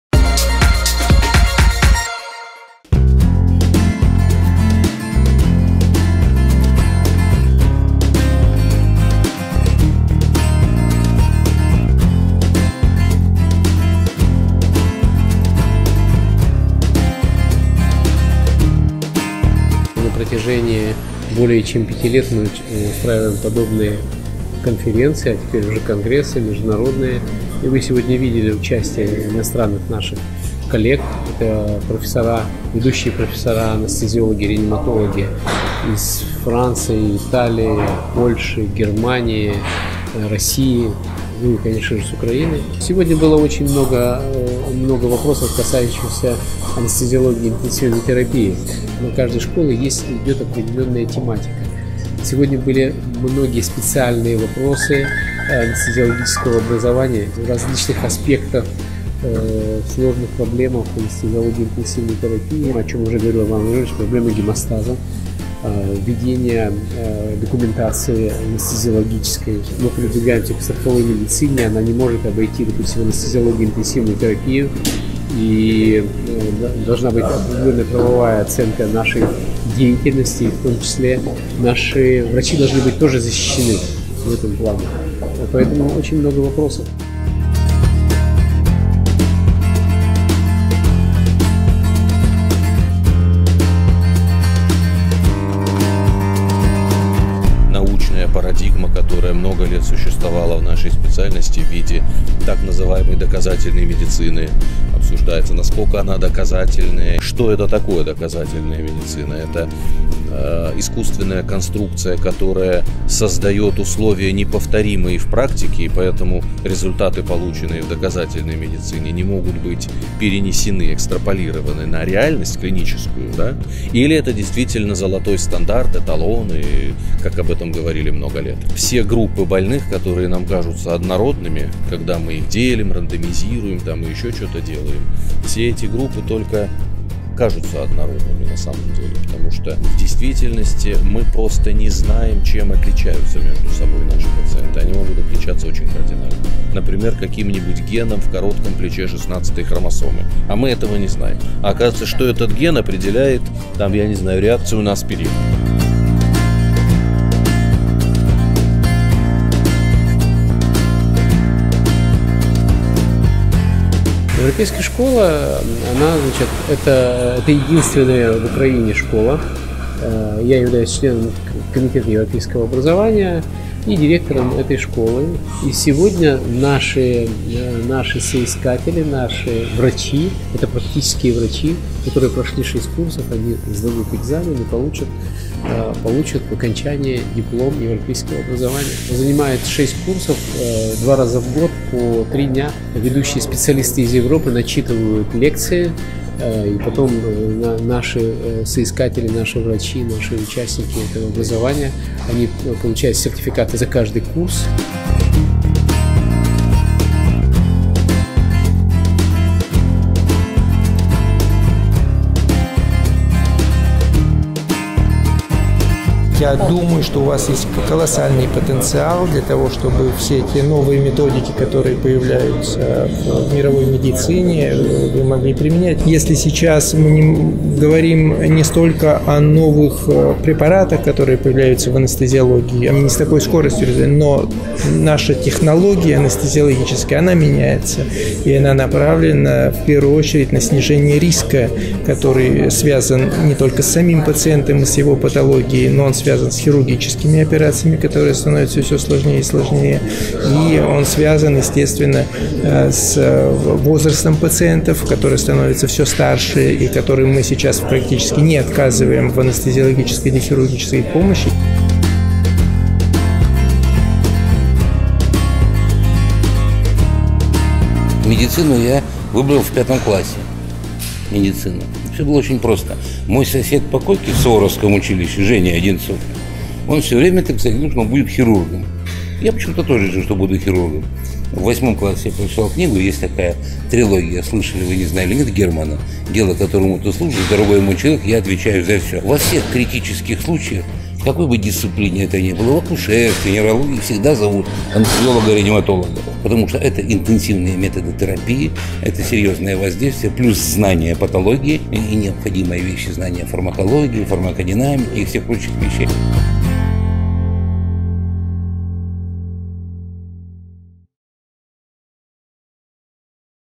Экскурсия анестезиологов в Odrex
Анестезиологи с мировым именем оценили Медицинский дом Odrex как проект, достойный внимания на международном уровне. После обзорного ознакомления с лечебным учреждением врачи пришли к заключению, что клиника удивляет своими новациями, компетентным по всем меркам персоналом и вселяет уверенность в  прочное будущее Odrex.
Команда анестезиологов, представленная в Odrex, - коллектив рассудительных врачей, взвешенно принимающих решения во время проведения операций, обдумывающих каждый свой шаг, настоящих мастеров науки сна. Это доктора, способные превратить медикаментозную терапию в отдых.